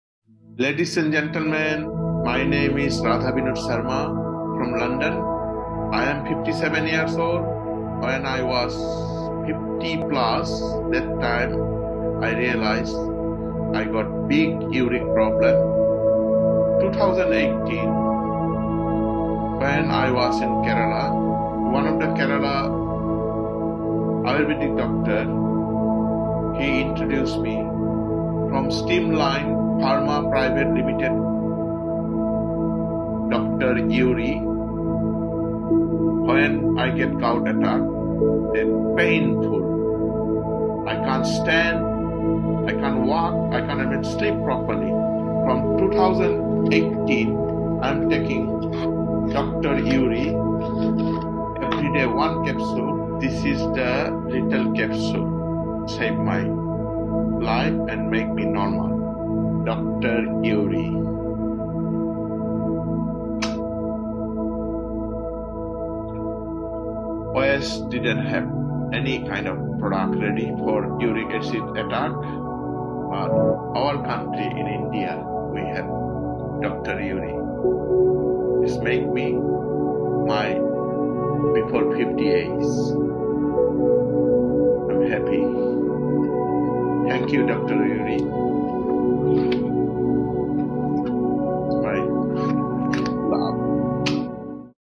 Satisfied Customer Series - Dr. Uri Testimonial
Here is a Story of Mr. Radha Vinod Sharma from UK.
Mr . Had severe  problem of Uric Acid .
In his visit to India in 2018 , Someone have suggested him about Dr. Uri .
After a successful treatment he has shared his views about our Products.

For More Case Studies, Please contact our sales team.